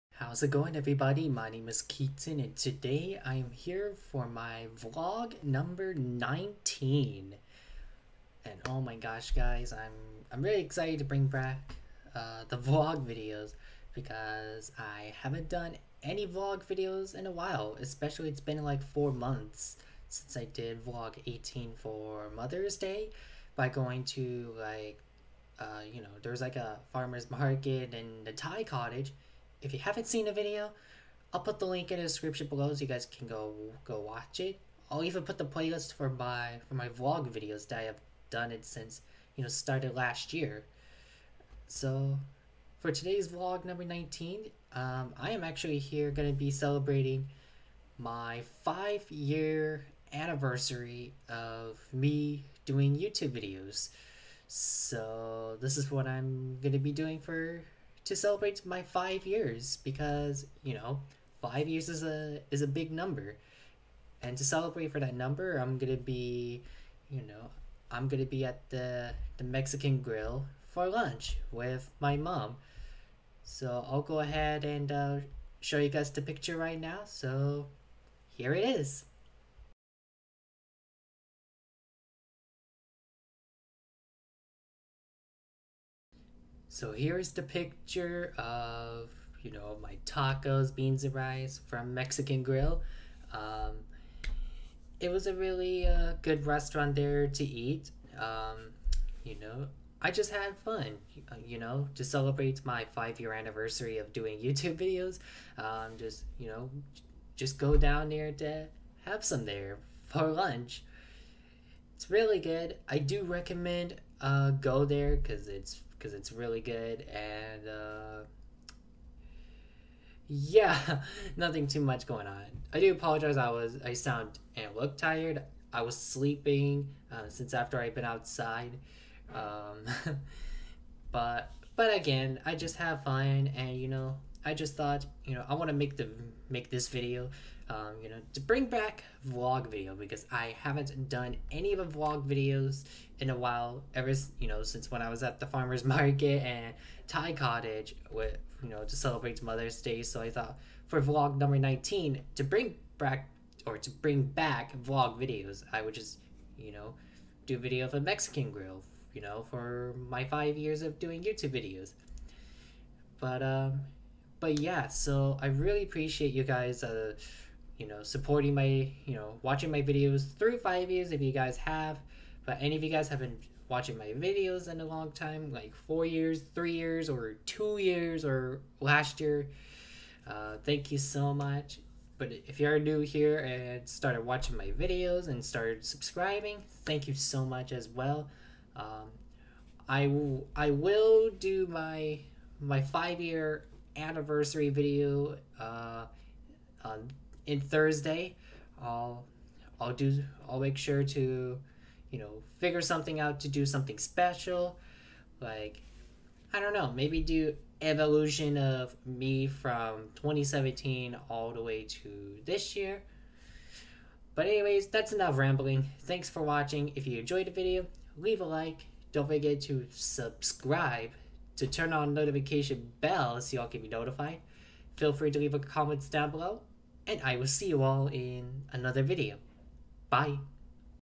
Vlog Nineteen - Mexican Grill
Welcome everybody to my Vlog Nineteen with me at the Mexican Grill to celebrate my 5 Year Anniversary of me making videos on YouTube which it is coming on Thursday, September 29, 2022! Thank you all so much for sticking with me watching my videos through long time if you all have and I'll make sure to do a video of my 5 Year Anniversary coming up soon